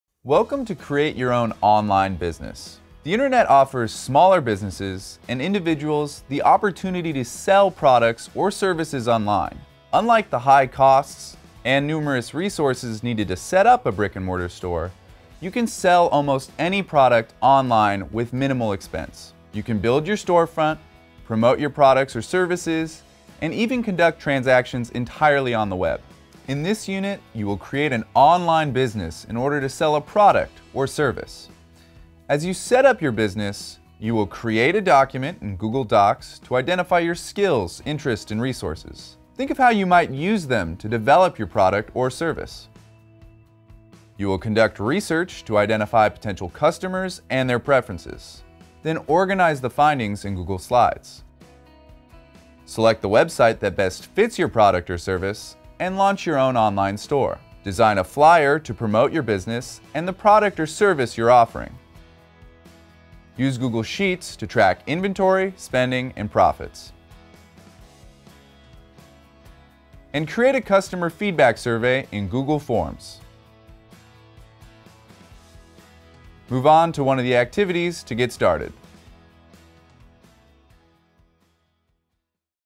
Introduction to Build Your Online Business
Build Your Online Business helps you prepare to launch a business to sell products or services on the internet using digital tools. In this video, you'll learn more about what this lesson involves.

0:00 - Sell products or services online with minimal expense
0:26 - What you will do in this unit as you set up your business